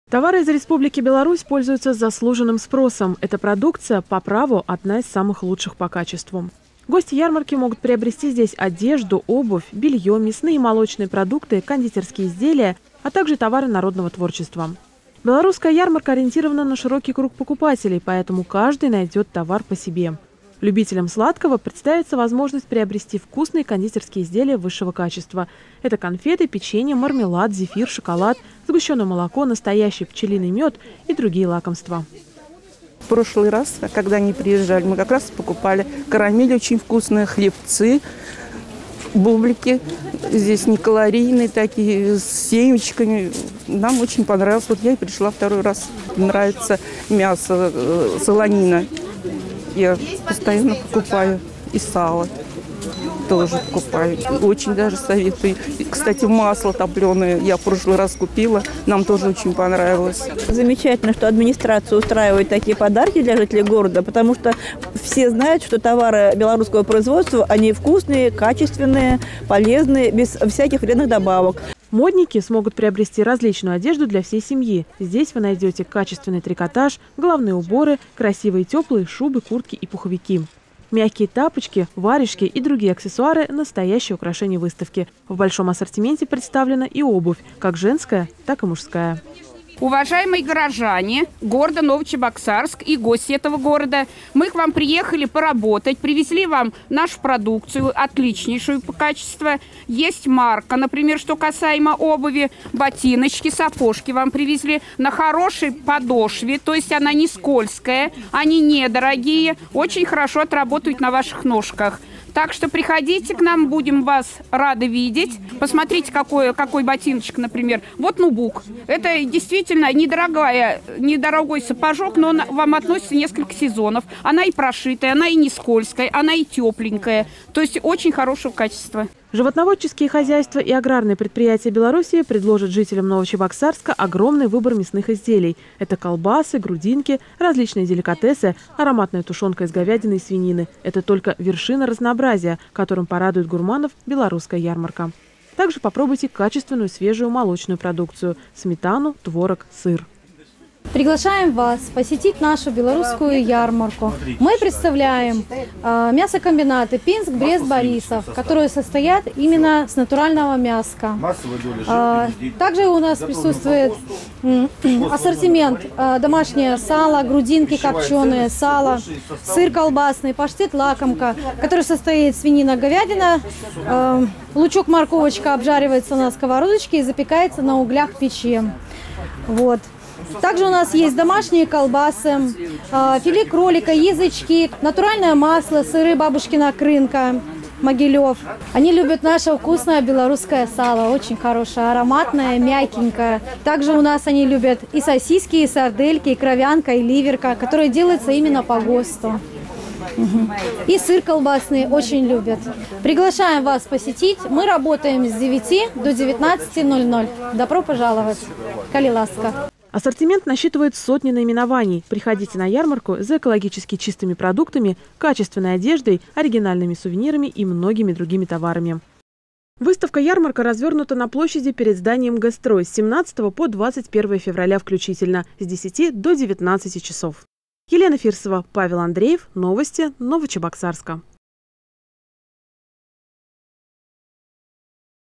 В Новочебоксарске с 17 по 21 февраля работает Белорусская ярмарка
С 17 по 21 февраля новочебоксарцы вновь могут приобрести товары Белорусской ярмарки. Вас ждёт широкий ассортимент разнообразных товаров, включающих в себя: одежду, продукты питания, товары хозяйственного назначения и многое другое.